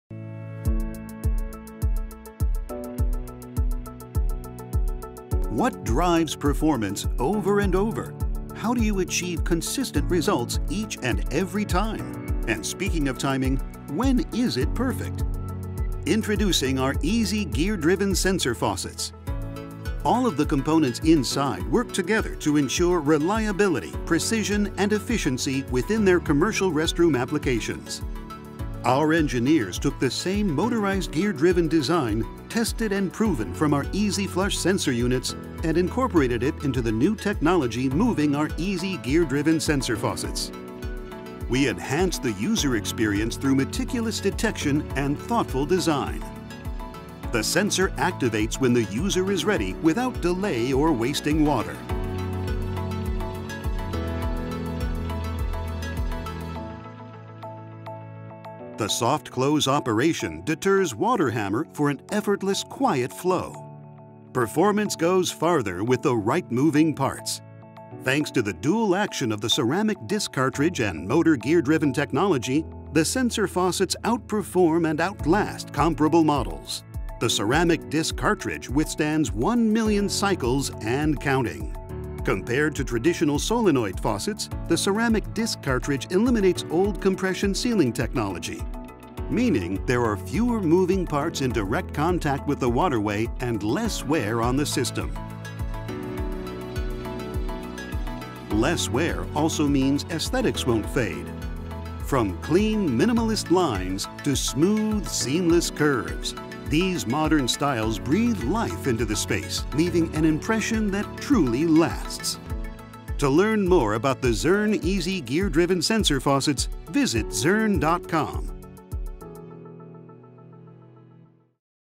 Zurn EZ Gear Driven Sensor Faucets
EZ Gear-Driven Sensor Faucets deliver reliability, precision, and efficiency to your commercial restroom. Thanks to the dual action of the ceramic disc cartridge and motor gear-driven operation, they outperform and outlast other faucets. They intuitively sense the user without delay or wasting water. Gear up for the next generation of sensor faucets with EZ Gear-Driven Technology.